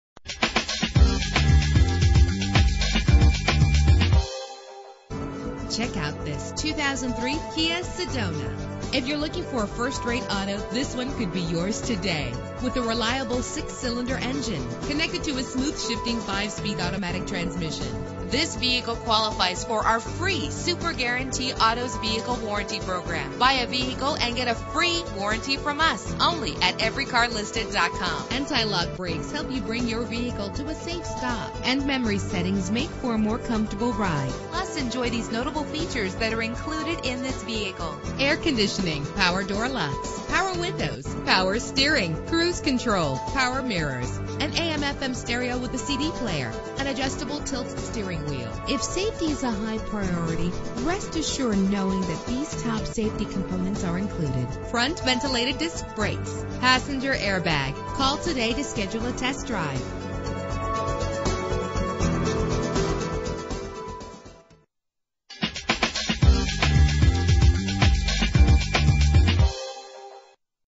2003 Kia Sedona - Lombard IL
Year: 2003
Make: Kia
Model: Sedona
Trim: EX
Engine: 3.5 liter 6 cylinder 24 valve
Transmission: 5-Speed Automatic
Color: Sage Green
Mileage: 101914
Address:
21 W 475 N Ave